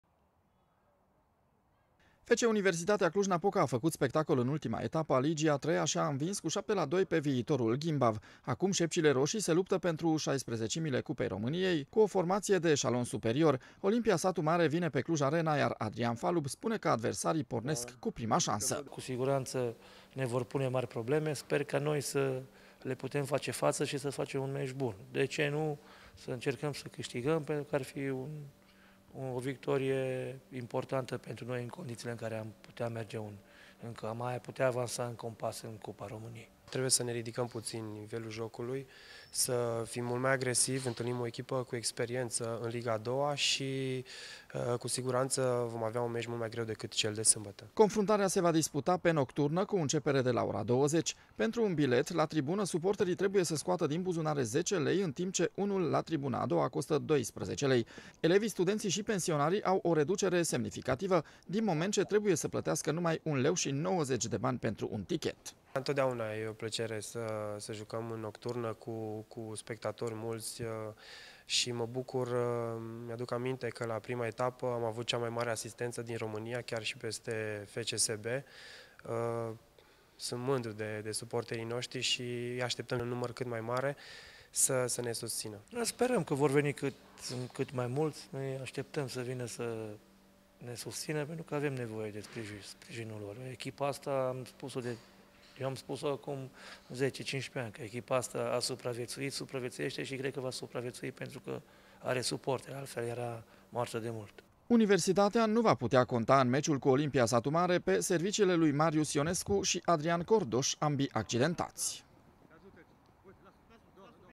Misiune dificilă pentru U
Fotbaliştii Universităţii Cluj-Napoca îşi continuă aventura în Cupa României. Şepcile Roşii se duelează în această seară cu Olimpia Satu Mare. Deşi adversarii vin din eşalon superior, băieţii lui Adrian Falub au un as în mânecă: “Jucăm pe nocturnă, unde am avut o asistenţă mai mare decât FCSB, aşa că aşteptăm sprijinul fanilor”, spun jucătorii clujeni.